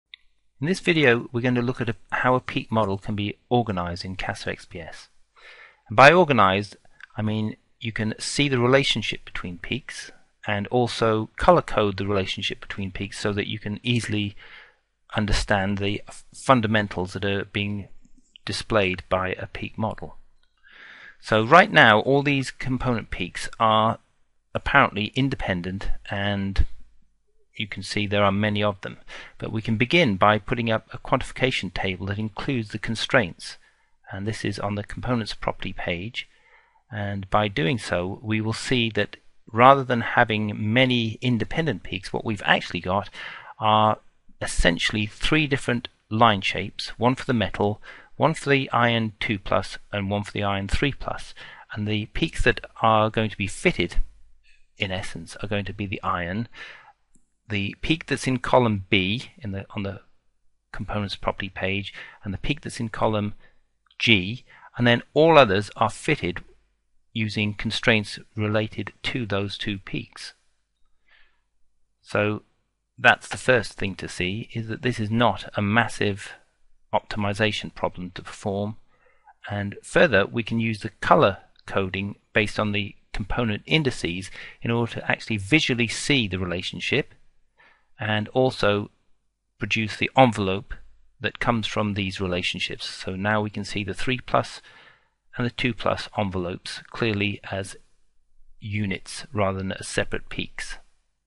Fe Part 1: Examining parameter constraints within a peak model in CasaXPS
Multiplet splitting is a feature of Fe 2+ and Fe 3+ oxidation states of iron. A peak model supported by theoretical studies is used to illustrate peak fitting techniques where the peak model is defined for one peak of a Fe 2p doublet.